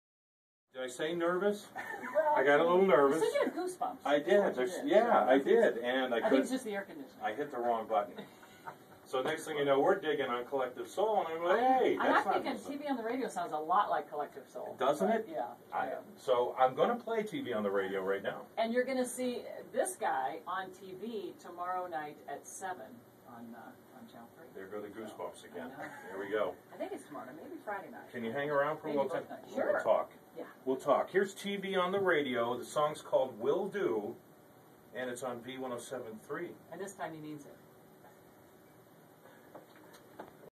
Robin Swoboda visits V107.3
Robin Swoboda from Channel 3 stops by to interview Rocco for a special on Cleveland DJs.